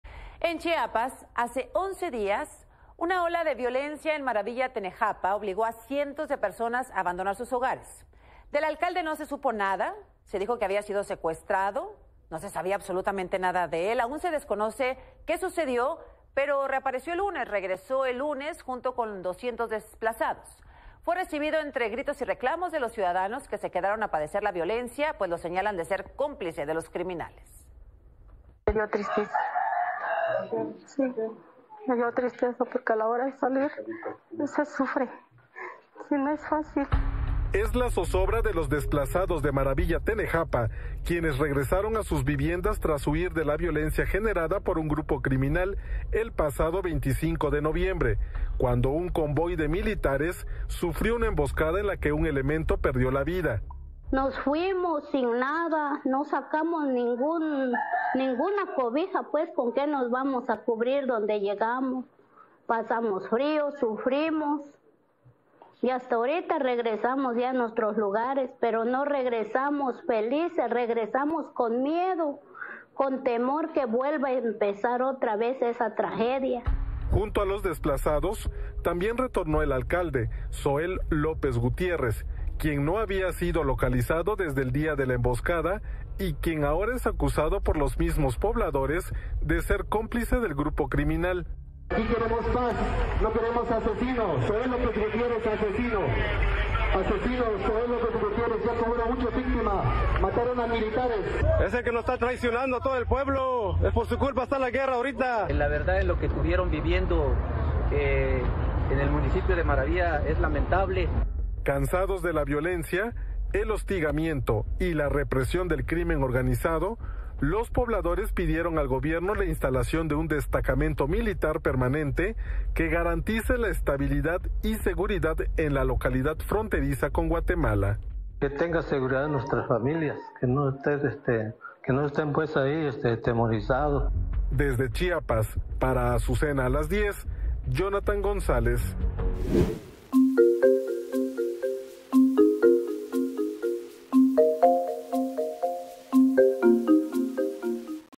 Alcalde de Maravilla Tenejapa reaparece tras 11 días de desaparición en Chiapas
Sin embargo, la reaparición del alcalde genera controversia, ya que algunos pobladores lo señalan como cómplice del grupo criminal que causó la violencia en la región